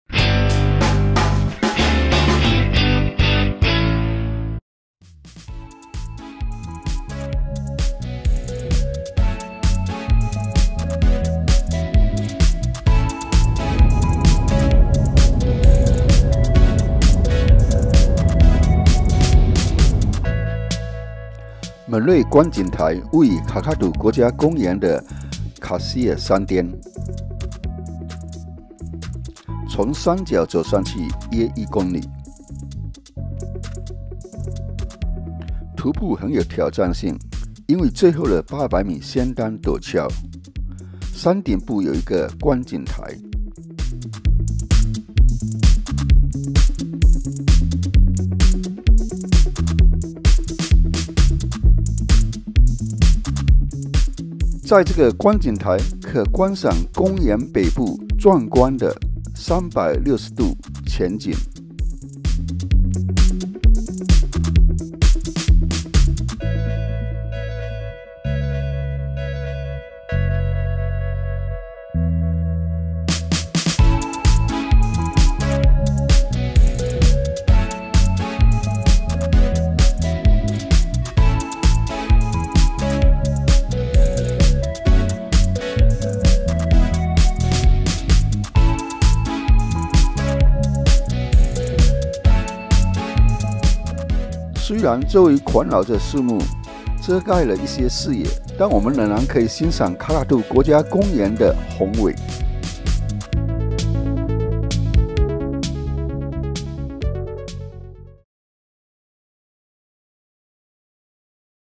Murray lookout, Kakadu National Park, Australia 澳大利亚卡卡杜国家公园穆雷 观景台
穆雷观景台位于卡卡杜国家公园的卡希尔山（Mount Cahill）巅。从山脚走上去约1公里。徒步很有挑战性，最后的800米相当陡峭。山顶部有一个观景台。在这个观景台可观赏公园北部壮观的360度全景。虽然周围环绕着树木，遮住了一些视野，但它仍然可以让你欣赏到卡卡杜国家公园的宏伟。

Murray Lookout is located on the top of Mount Cahill in Kakadu National Park. We walked about 1 kilometer to reach the top of the mountain. Hiking is very challenging, and the last 800 meters are quite steep.There is a look out at the top of the mountain. At this lookout, you can enjoy a spectacular 360-degree panoramic view of the north of the park.